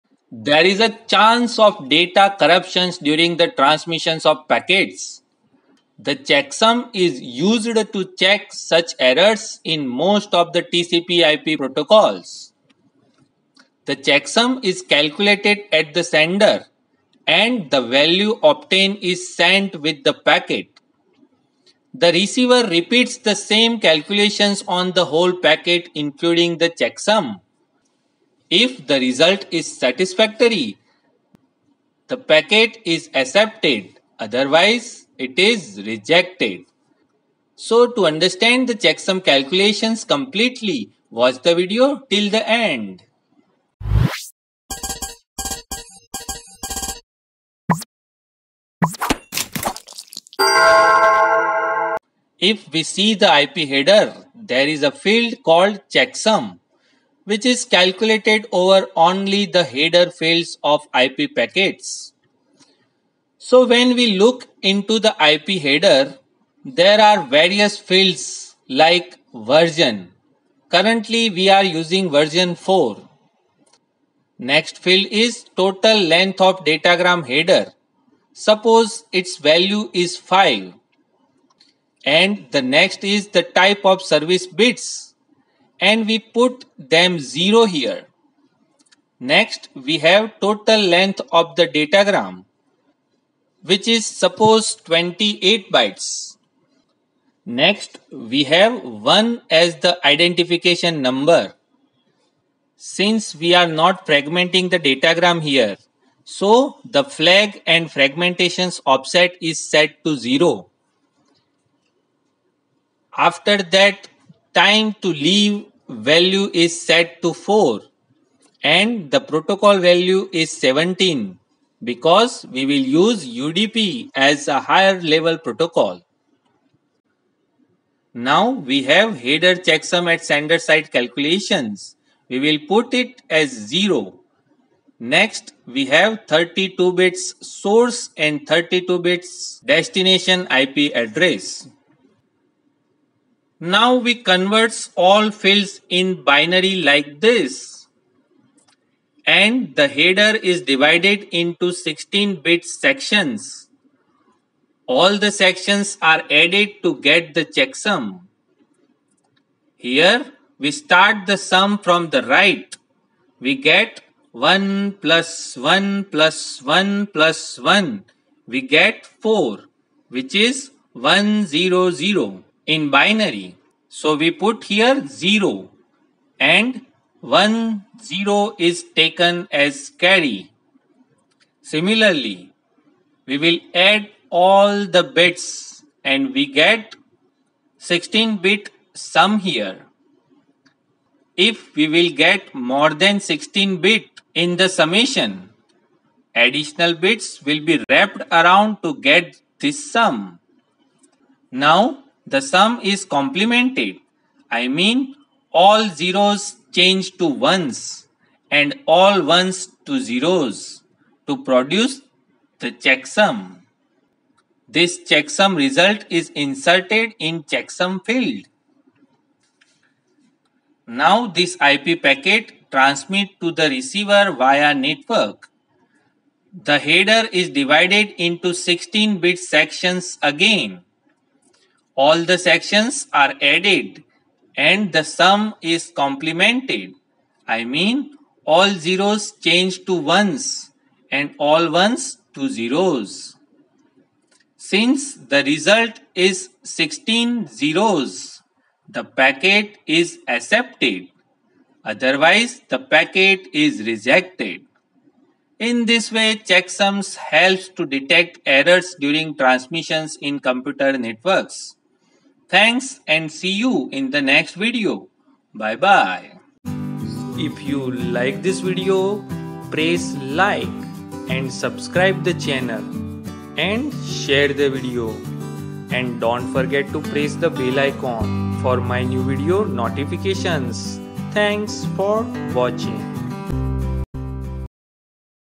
How Checksum Calculated in IP
There is a chance of data corruption and error during the transmission of packets.  The checksum is used to check such errors in most of the TCP IP protocols. The checksum is calculated at the sender and the value obtained is sent with the packet. The receiver repeats the same calculation on the packet including the checksum. If the result is satisfactory, the packet is accepted; otherwise, it is rejected. This video explains how the checksum is calculated at the sender side and verified at the receiver side with example.